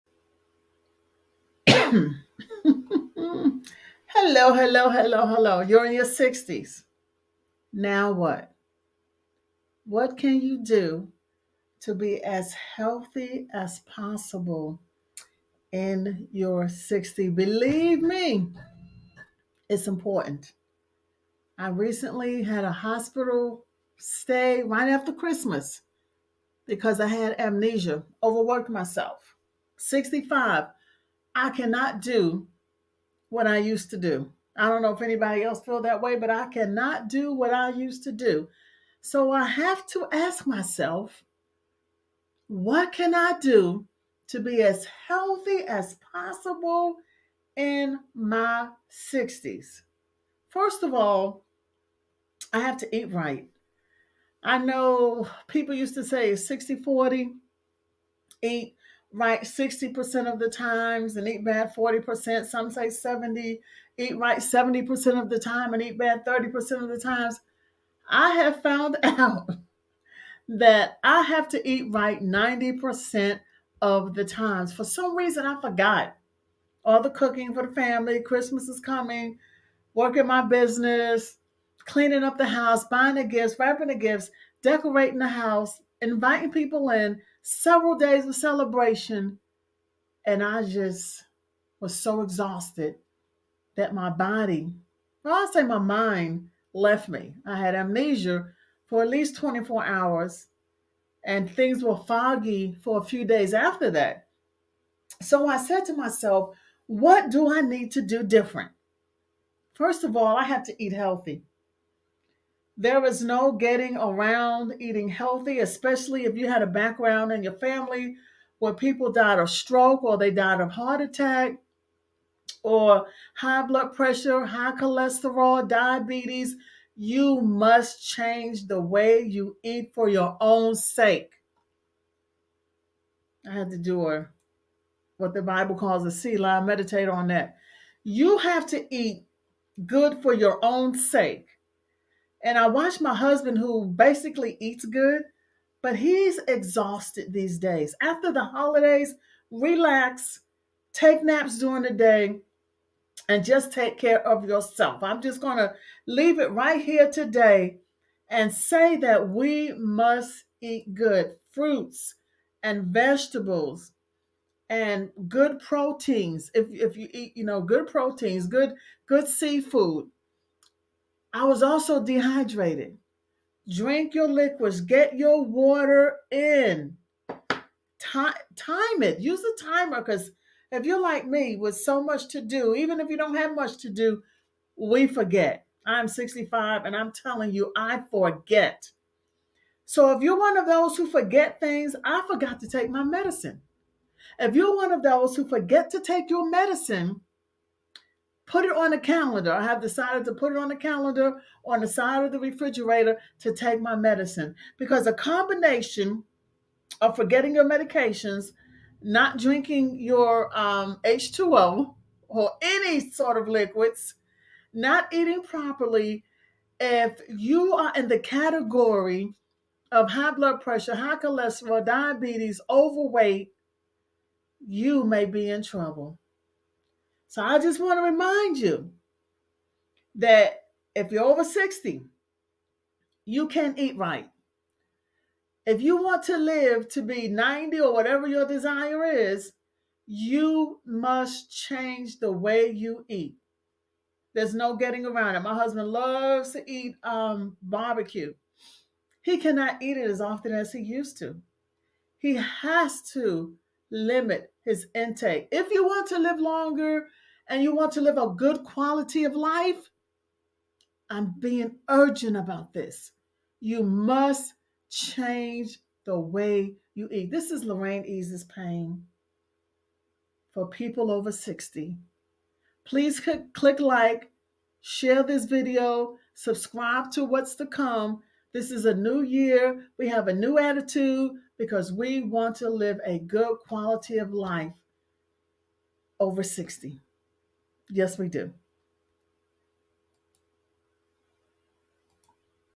What can you do to be healthy after 60?
Unlock wellness after 60: Essential Tips for a Healthy Lifestyle. Thrive in your golden years.